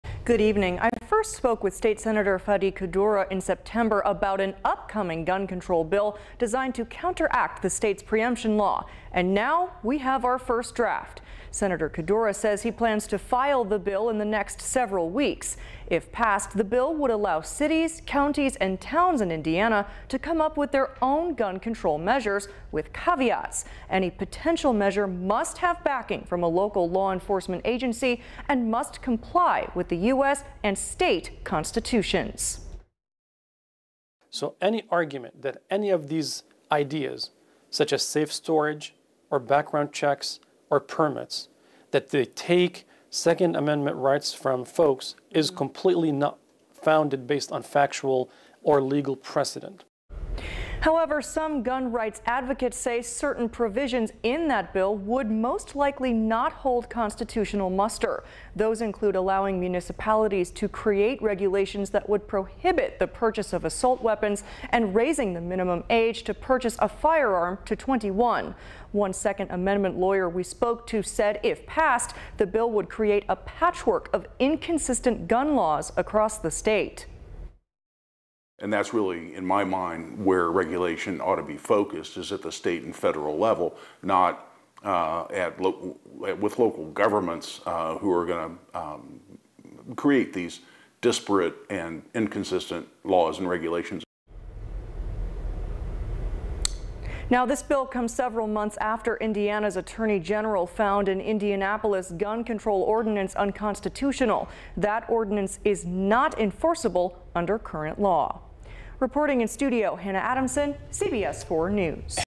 First draft of Indiana gun control bill introduced
Indiana State Sen. Fady Qaddoura said a new bill he plans to file in the next few weeks seeks to counteract the state’s pre-emption law — a law that’s been on the books since 2011 and prohibits Indiana cities, counties, and towns from creating their own gun control regulations.